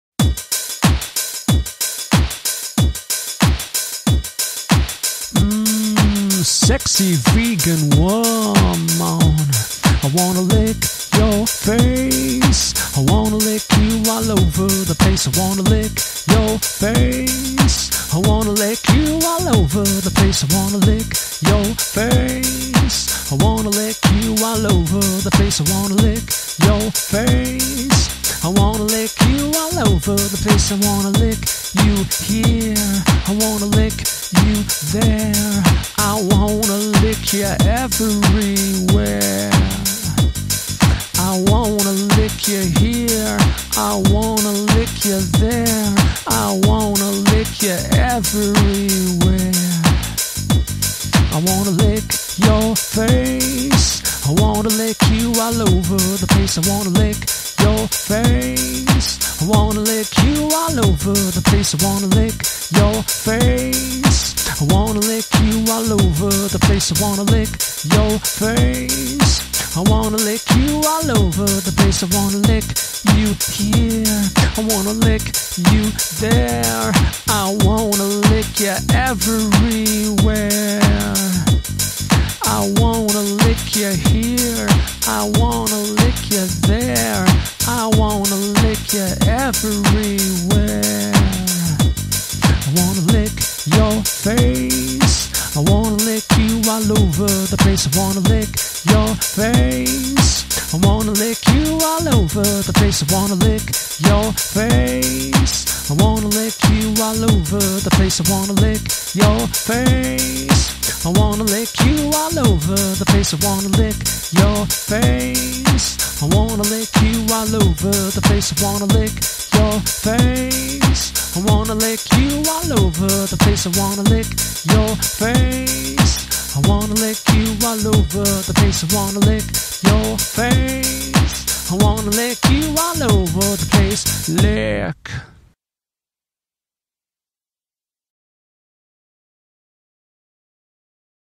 I Wanna Lick Your Face
My vegan superhero friend was joking around on Facebook and she said, "I want to see you in front of a giant pic of me, licking my face." Surprise! I hope you like it!